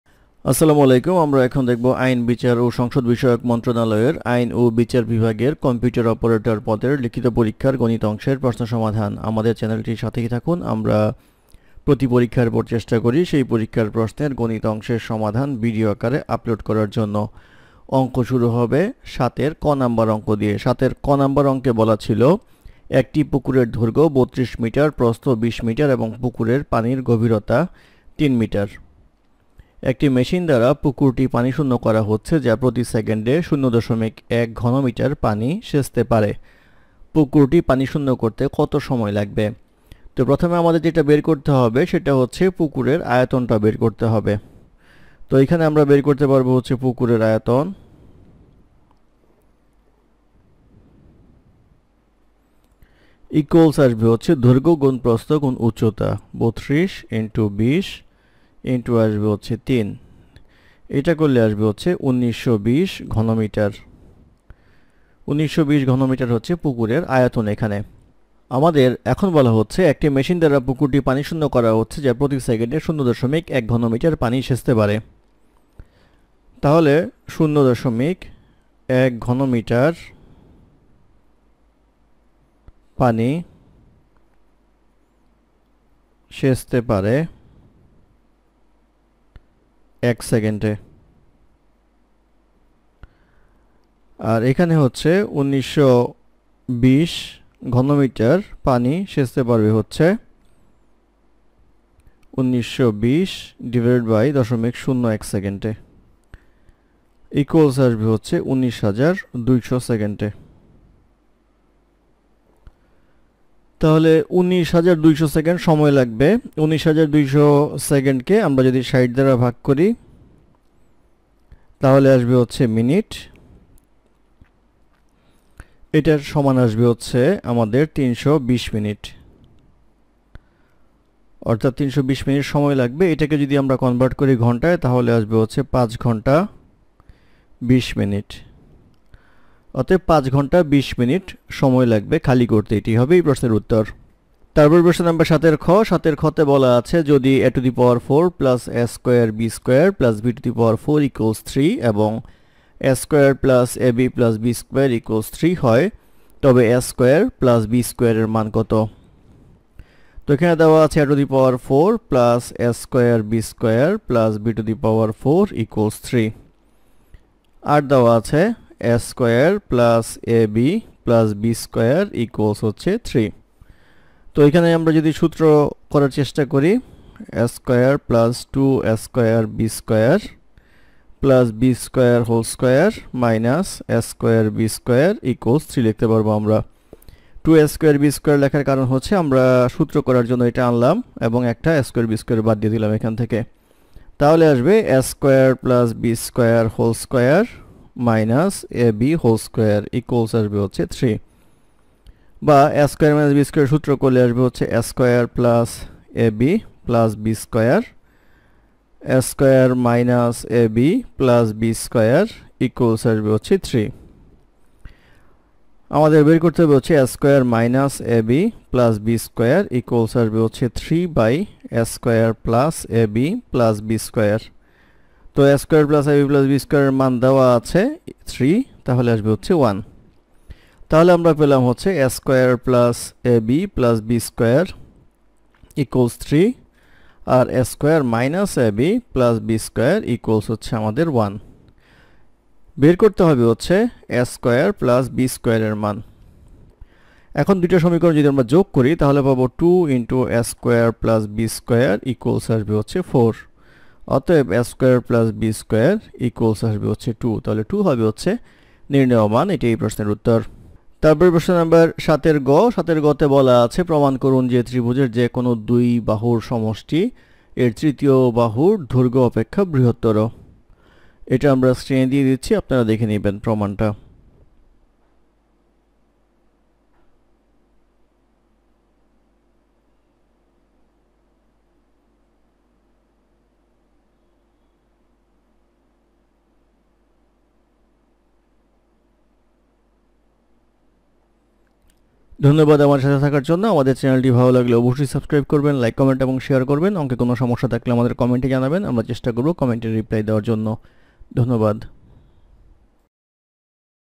আইন ও বিচার বিভাগ পদ: কম্পিউটার অপারেটর Written Math Solution Exam Date: 25-05-2024
আইন, বিচার ও সংসদ বিষয়ক মন্ত্রণালয়
আইন ও বিচার বিভাগ
পদ: কম্পিউটার অপারেটর
৭। ক) একটি পুকুরের দৈর্ঘ্য ৩২ মিটার, প্রস্থ ২০ মিটার এবং পুকুরের পানির গভীরতা ৩ মিটার। একটি মেশিন দ্বারা পুকুরটি পানিশূন্য করা হচ্ছে যা প্রতি সেকেন্ডে ০.১ ঘনমিটার পানি সেচতে পারে। পুকুরটি পানিশূন্য করতে কত সময় লাগবে?
৭। খ) যদি a4 + a2b2 + b4 = 3 এবং a2 + ab + b2 = 3 হয়, তবে a2 + b2 এর মান কত?
৭। গ) প্রমাণ করুন যে, ত্রিভুজের যে কোনো দুই বাহুর দৈর্ঘ্যের সমষ্টি এর তৃতীয় বাহুর দৈর্ঘ্য অপেক্ষা বৃহত্তর।